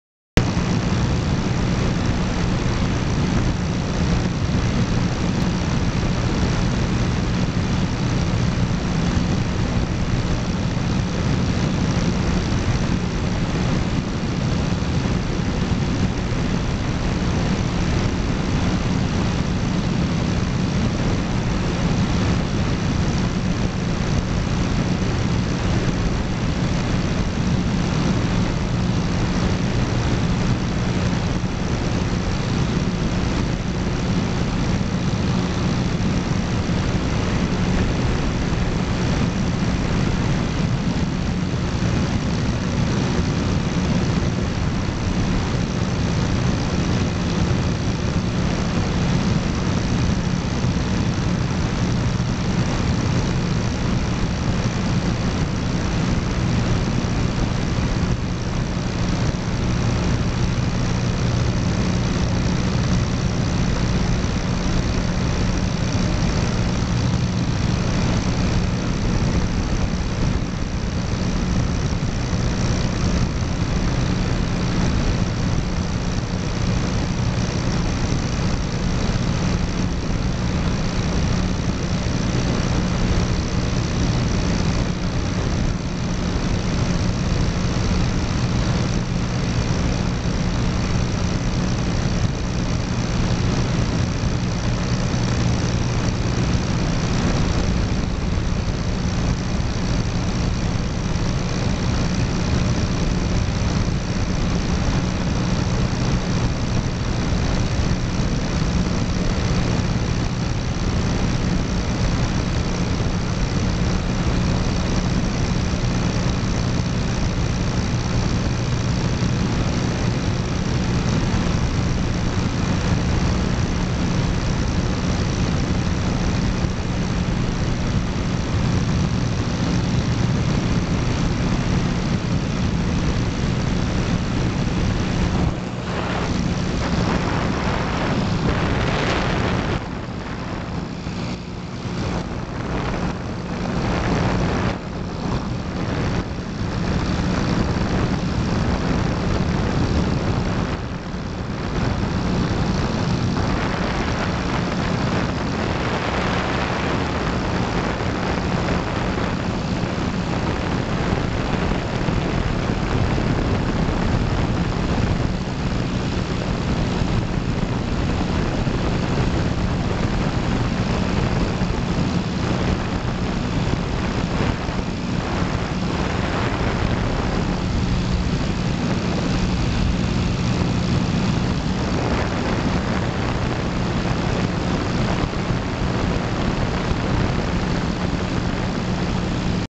ビクトリアフォールズ遊覧飛行
世界三大瀑布の一つ、ビクトリアの滝。上空からみるとジグザグの渓谷がよくわかります。